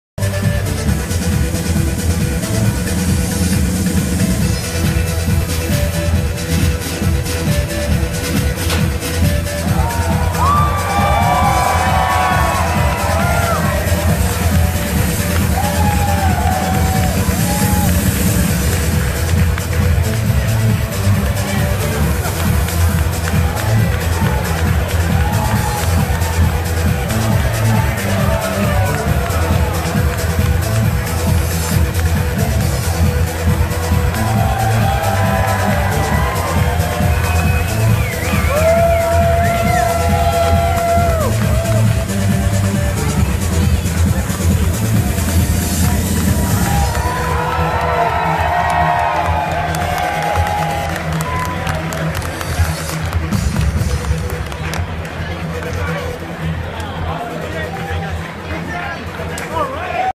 Video: Minnesota United's new kits unveiled through curtain drop and the smokescreen.
By: Andy Greder - Writer for @PioneerPress
Published on: February 17, 2017